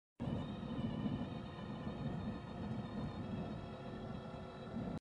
Lions Nathan Williamson
Rover POV of the Vumbi lion pride resting on grassland.